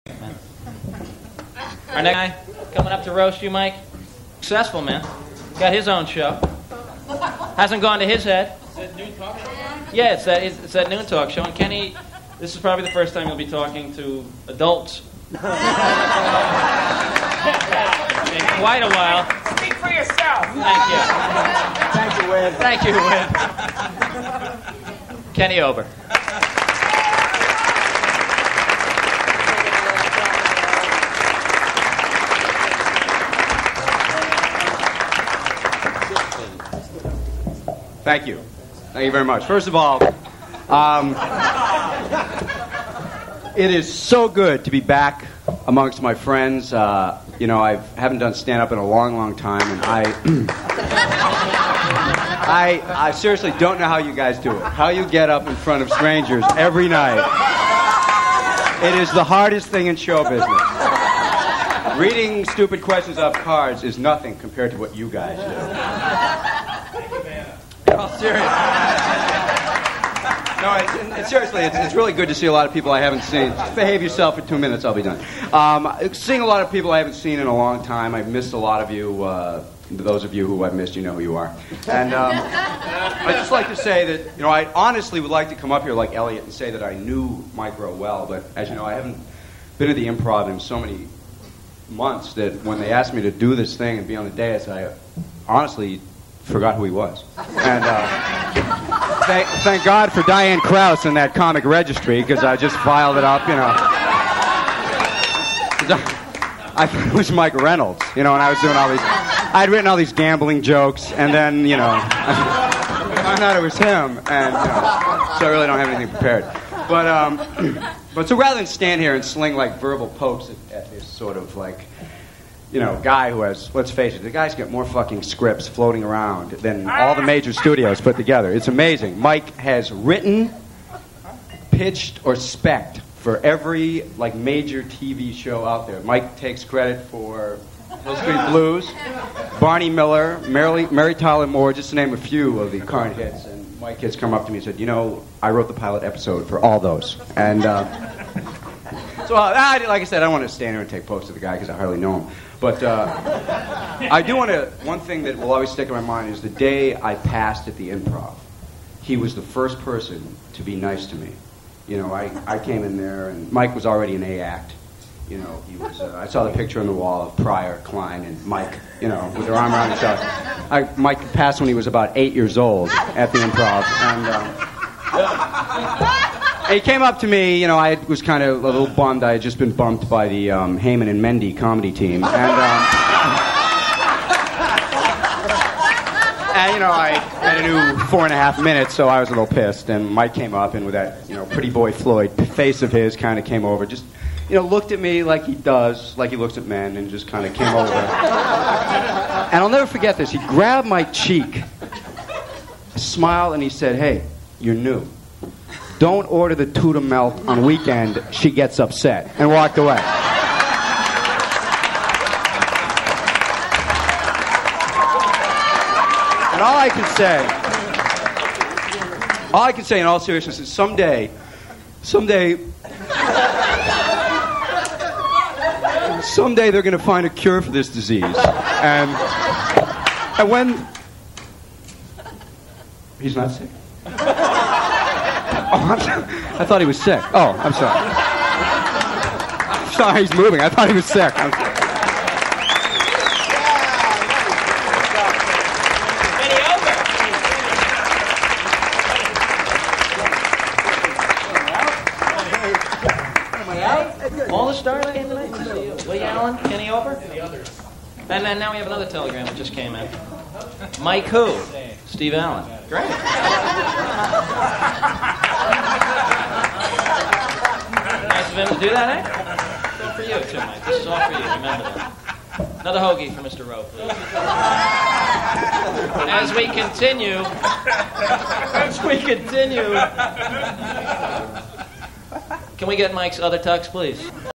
Ken Ober Roasts Mike Rowe
Ken Ober Home Movie 2: Years ago I was packed and ready to move to "Hollywood" for my first big-time writing job. In honor of this Michael Patrick King put together a roast at the lovely Skyline Motel on 10th Ave. in New York City. This is Ken Ober's tribute to me.